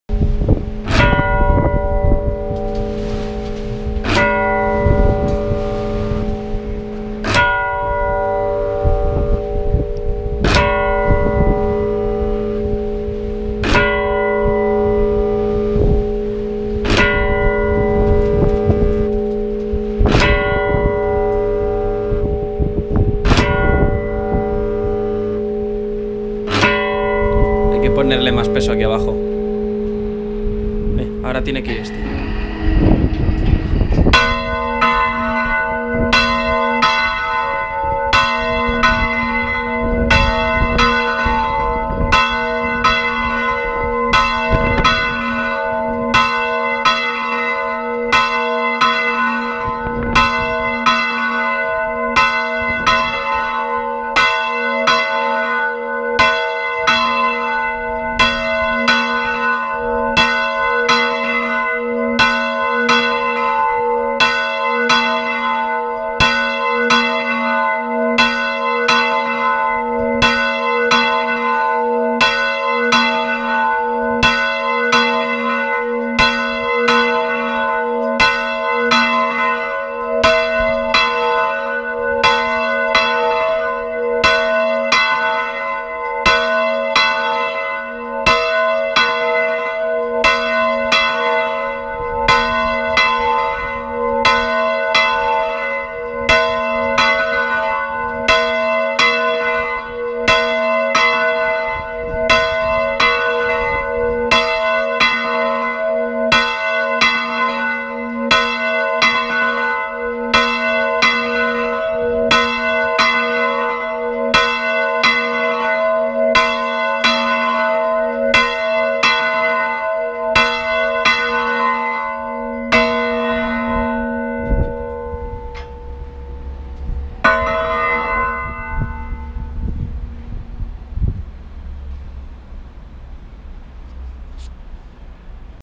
Repique y volteo de campana Parroquia de San Marcos de Madrid
Extraño sistema de repique semi-restaurado  y volteo de campana con yugo y motor del año 1939 funcionando.
Las campanas están fundidas por Constantino de Linares en Carabanchel Bajo a excepción de la que repica, que se fundió por el antiguo fundidor de Quintana de Alcalá de Henares en el año 1979.